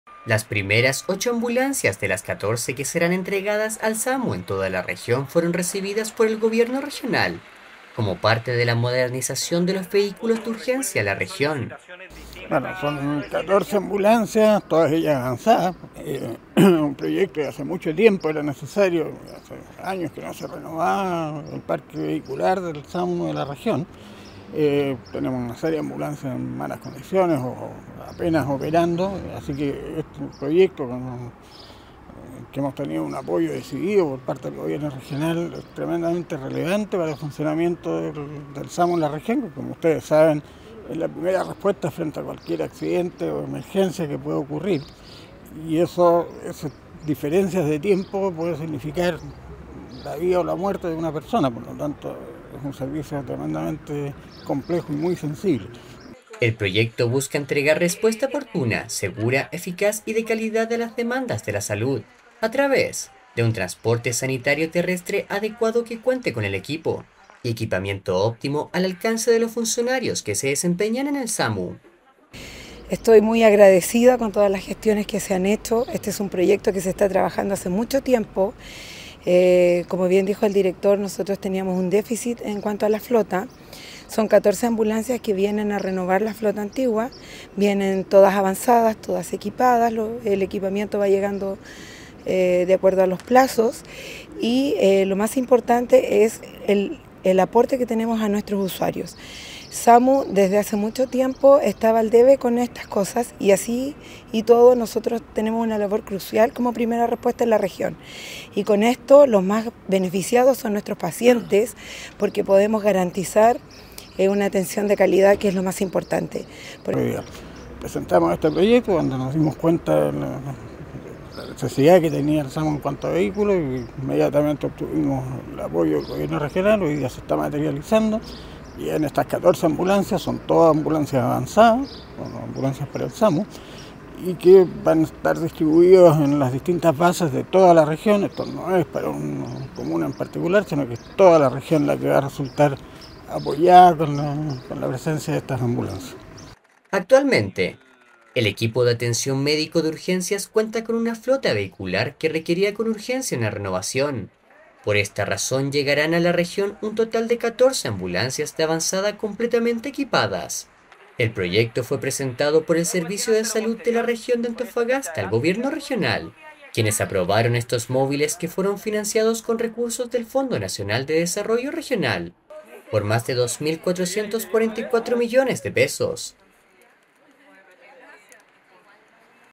01 oct 2024 Distribuirán 14 ambulancias para trabajo del SAMU en la región
01 oct 2024 El Gobierno Regional de Antofagasta recibió las primeras ocho ambulancias, de un total de 14 destinadas al servicio de atención médica de urgencia en la región, como parte de un proyecto de modernización de su flota vehicular... Estos móviles, completamente equipados, buscan mejorar la respuesta oportuna y segura de las demandas de salud, asegurando un servicio sanitario ante urgencias